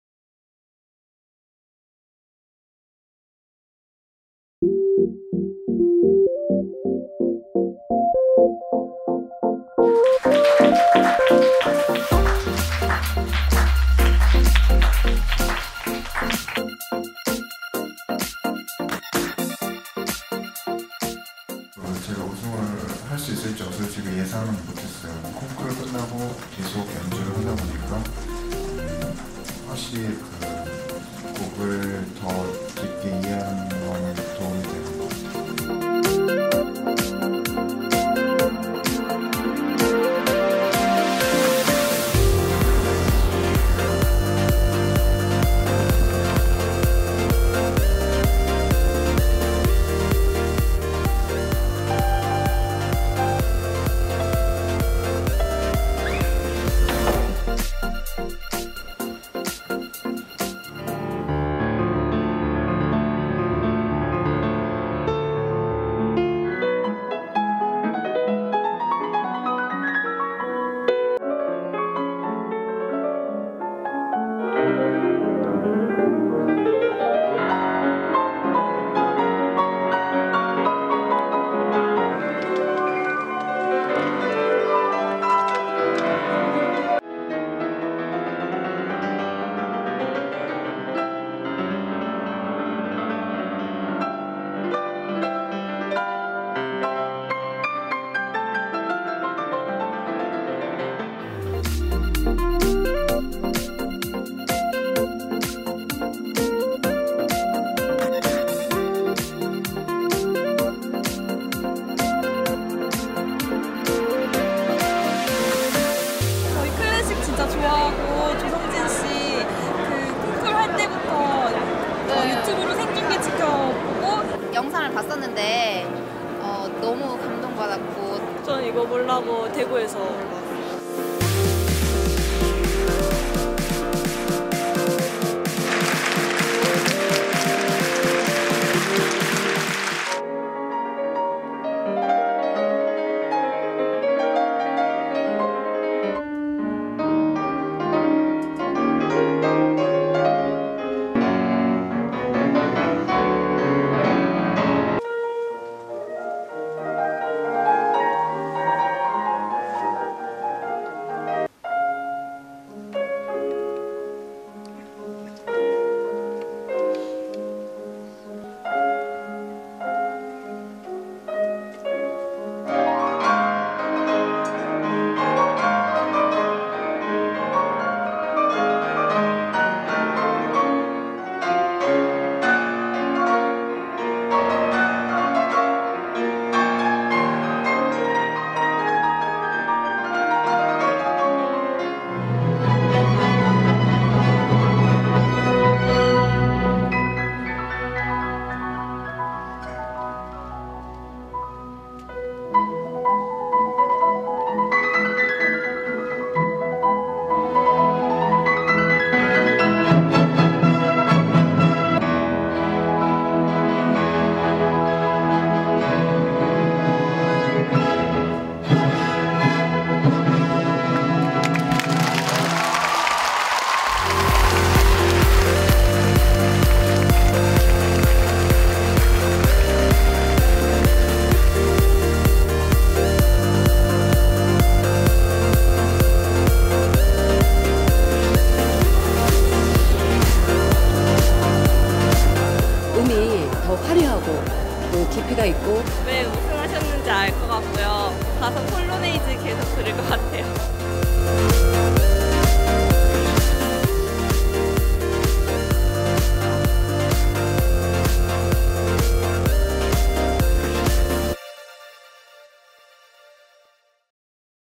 쇼팽 피아노 콩쿠르 갈라 콘서트 우승자 갈라 콘서트 (조성진)
쇼팽 피아노 콩쿠르 갈라 콘서트 우승자 갈라 콘서트 2016.2.2 예술의전당 콘서트홀
Chopin Piano Competition Gala Concert In SEOUL